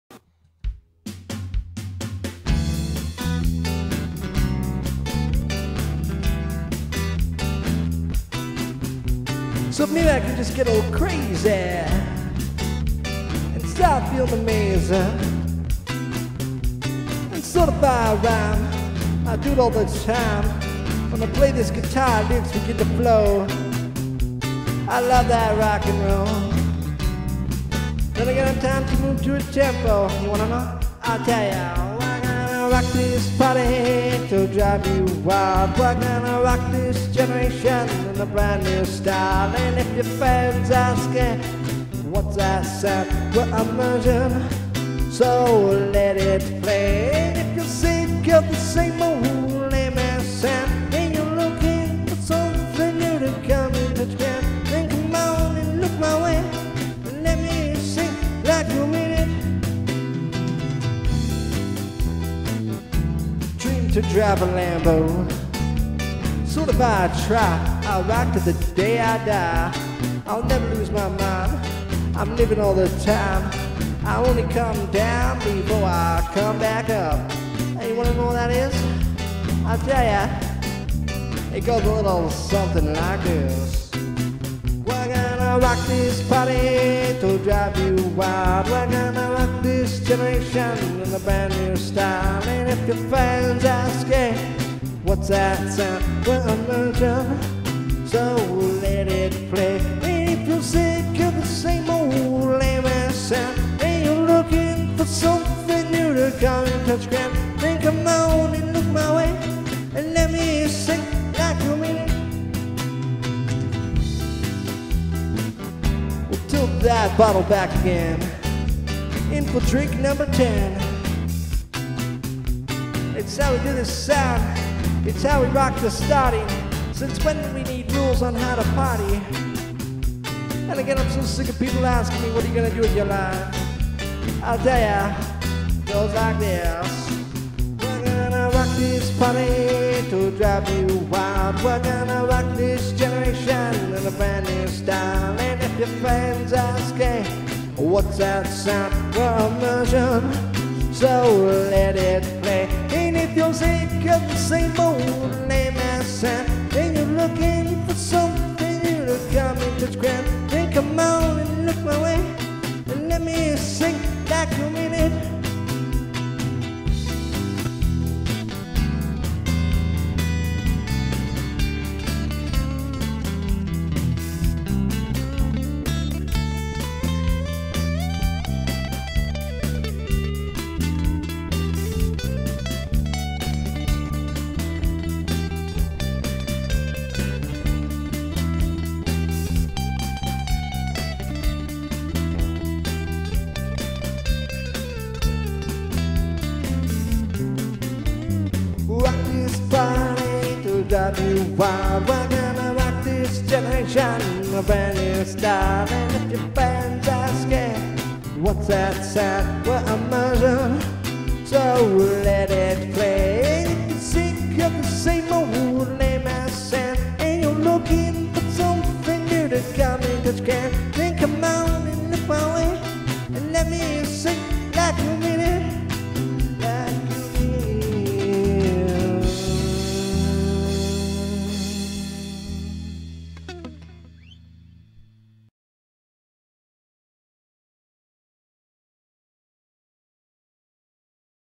Rock This Generation
Back Woods and Sharko rocking some jazz punk think Sublime style!